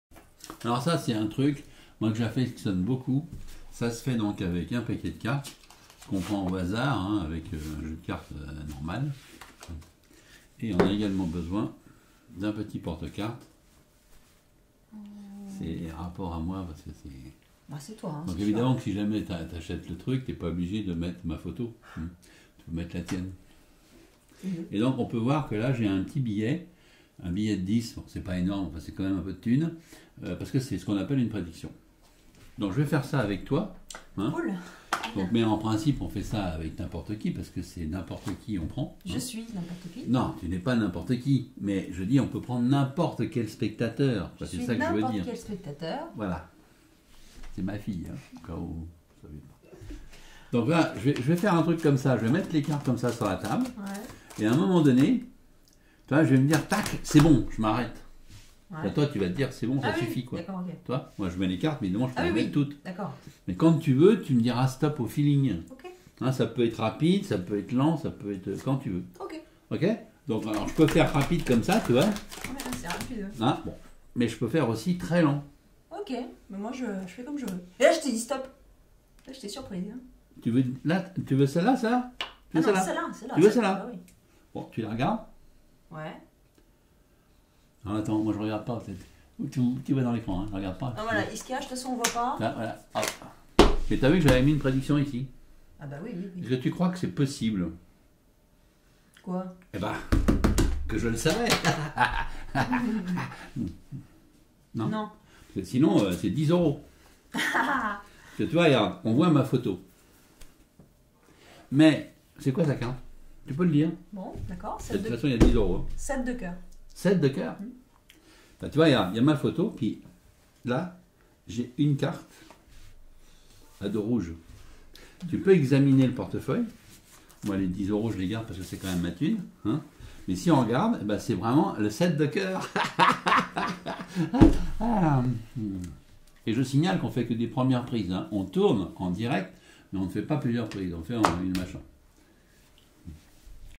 Mini Duvivier Wallet - Le Ken de Duvivier
Un Himber Wallet avec le petit plus de Duvivier qui le rend examinable - 9 routines (dont celle-ci) expliquées sur le DVD